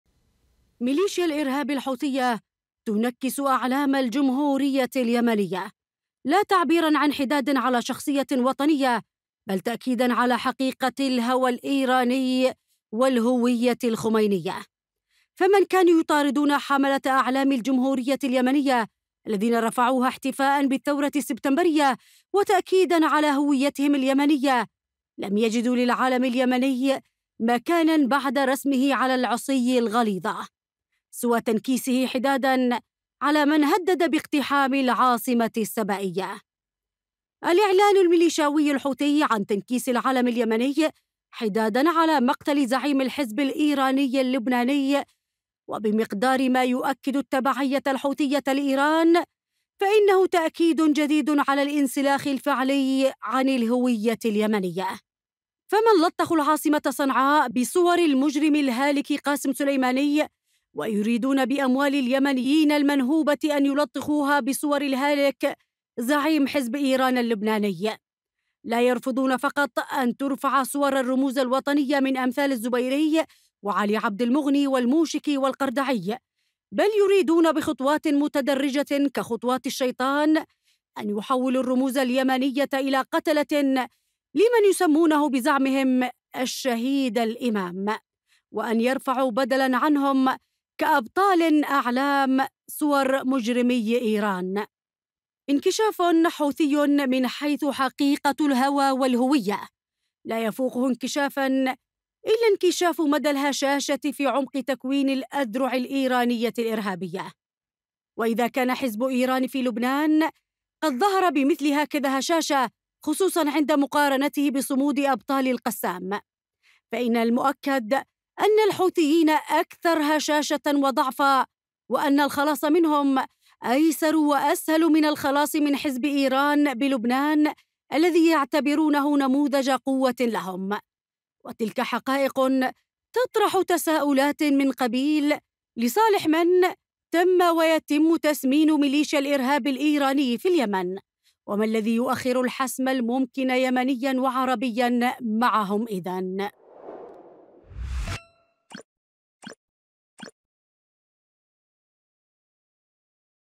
الإعلان الحـ..ــو ثي عن تنكيس العلم اليمني تأكيد على الهوى الإيراني والهوية الخمينية
قناة_سهيل# ‪